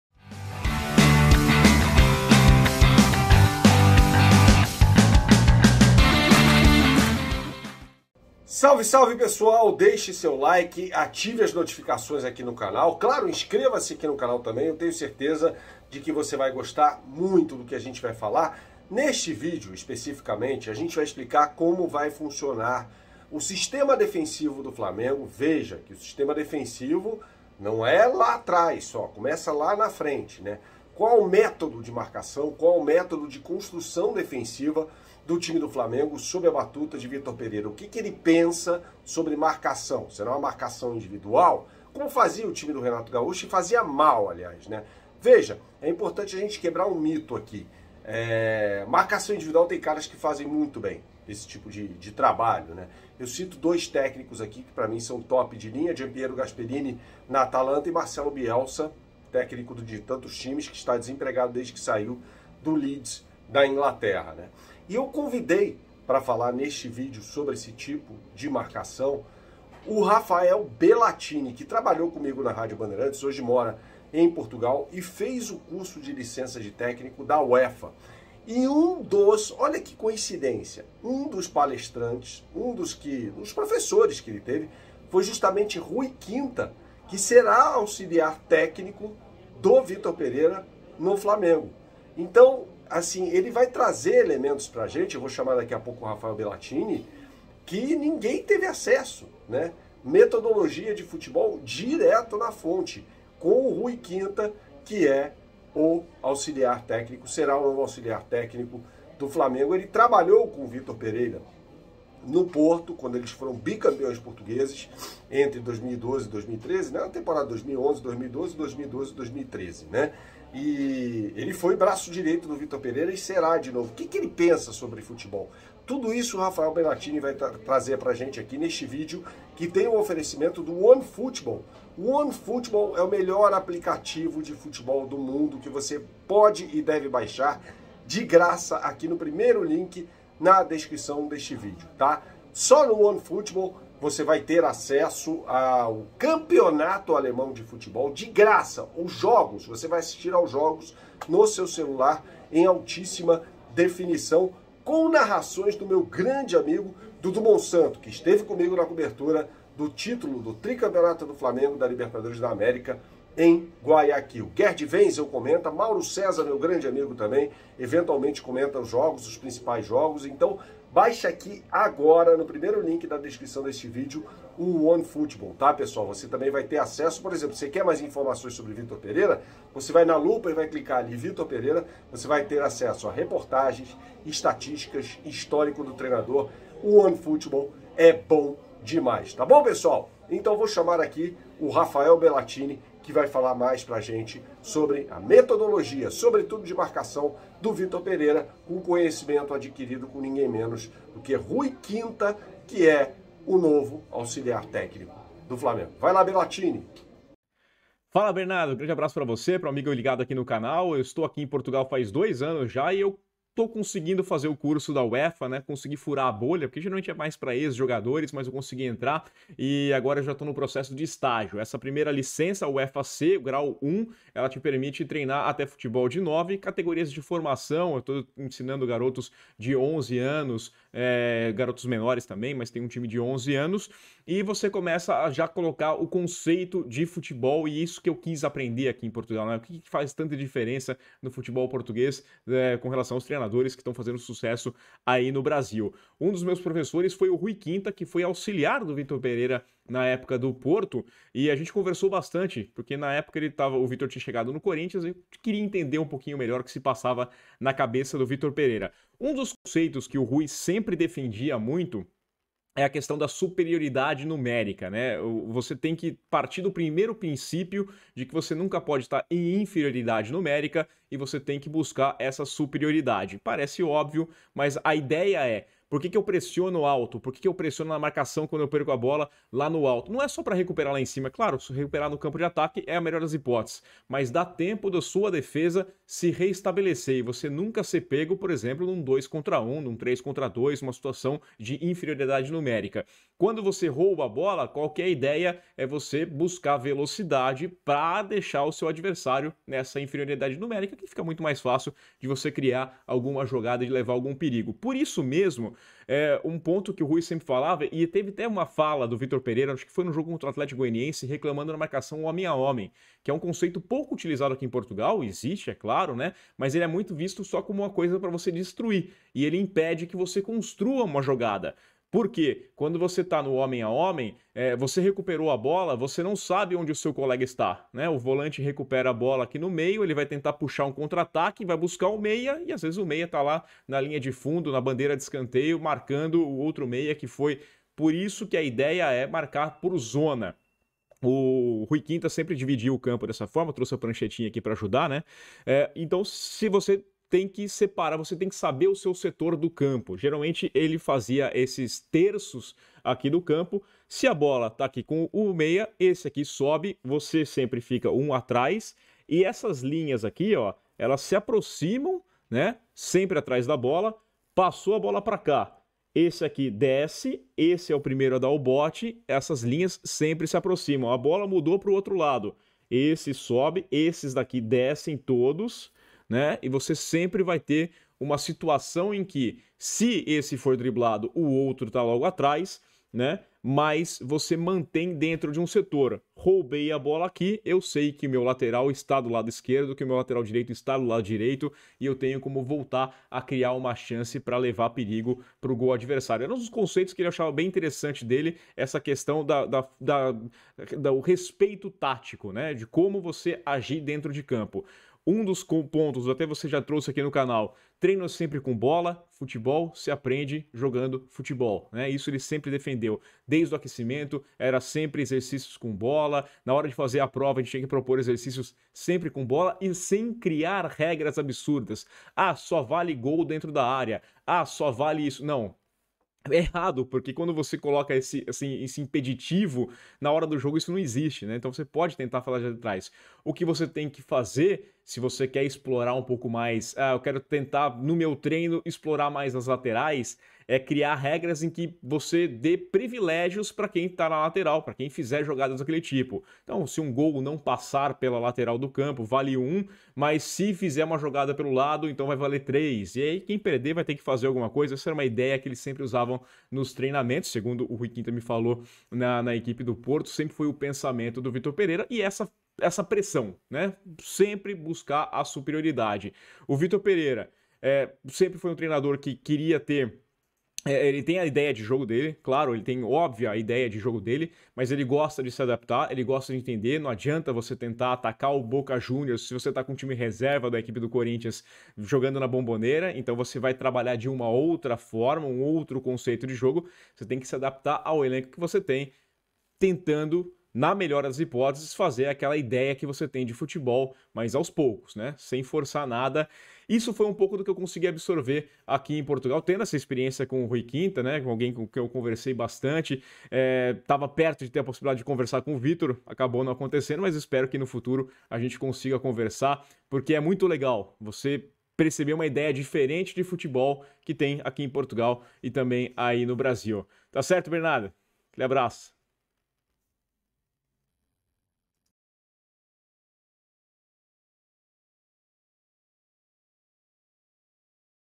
Vitor Pereira usará marcação por setor no Flamengo, diz aluno de Rui Quinta, novo auxiliar do clube
Veja como vai funcionar o sistema defensivo do Flamengo com um aluno de Rui Quinta, que será o novo auxiliar técnico de Vitor Pereira.